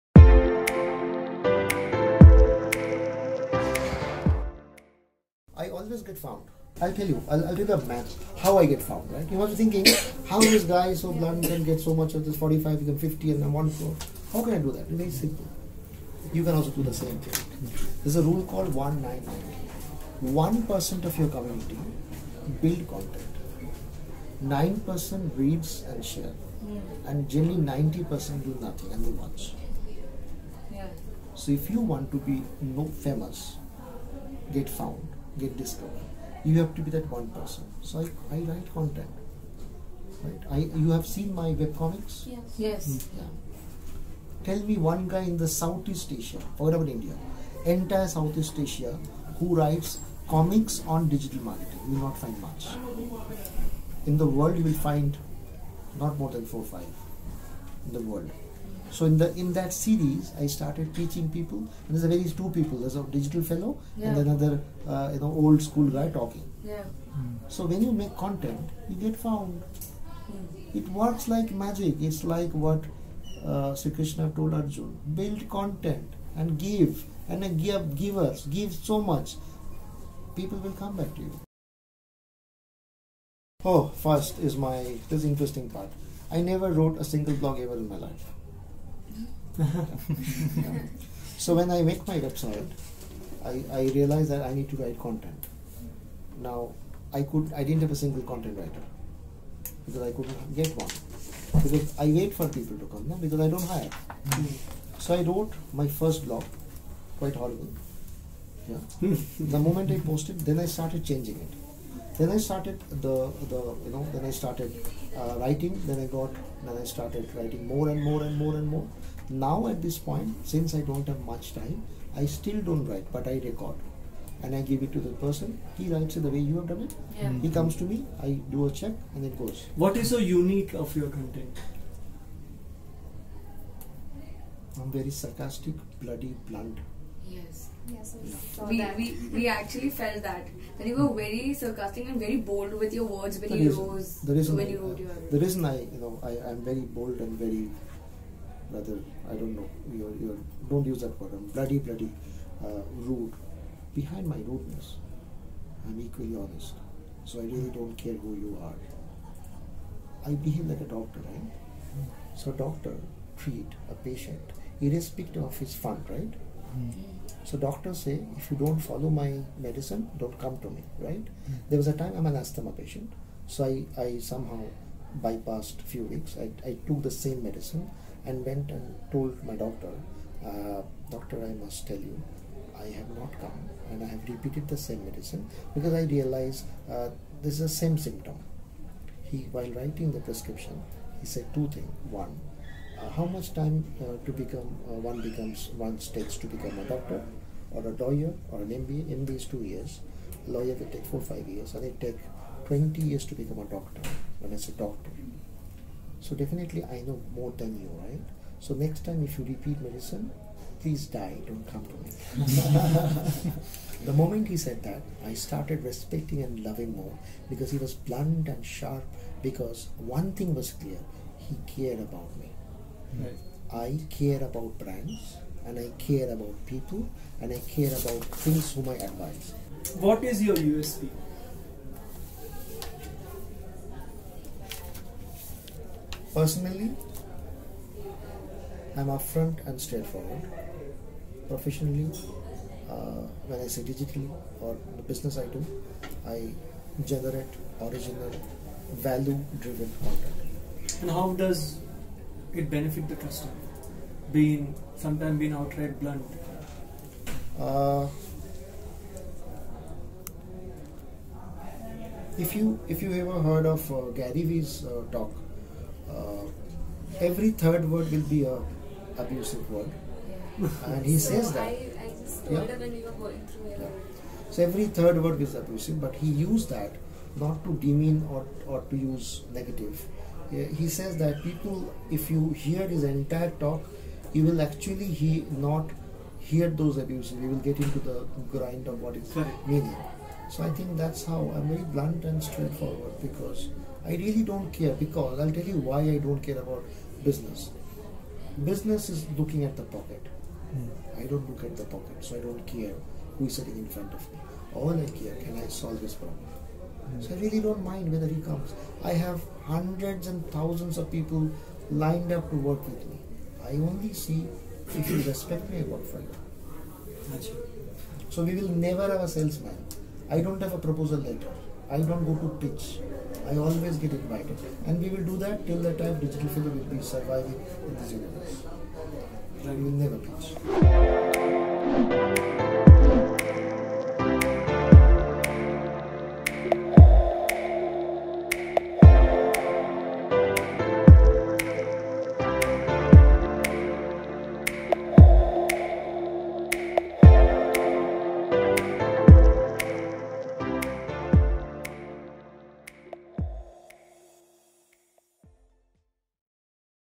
What is Marketing Model 1-9 -90 ? | Subhobroto Chakroborty
There is a Rule called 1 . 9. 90
1% of your community build content 9% reads and shares And generally, 90% do nothing and they watch so if you want to look famous get found get discovered you have to be that 1% so I write content You have seen my webcomics Tell me, one guy in south-east Asia forget about India entire south-east Asia who writes comics on digital marketing You'll not find much
In the world you'll not find not more than 4 – 5 in the world So in that series I started pitching people There is two people the digital fellow and another old-school guy talking
so when you make the content you'll get found.

A Premium Business Consulting Firm, a value-driven and User-centric approach to transforming brands and individuals digitally.